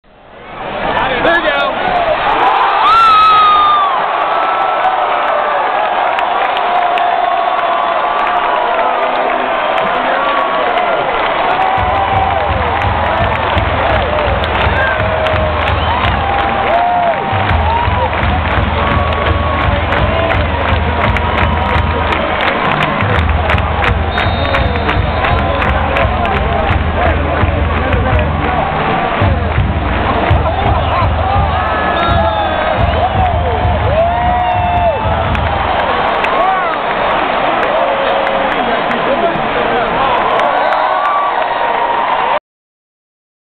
Touchdown Gators! Florida vs South Carolina 2008
Florida defeats Spurrier and South Carolina in the Swamp on November 15th, 2008. This touchdown made it on Sportscenter's Top 10!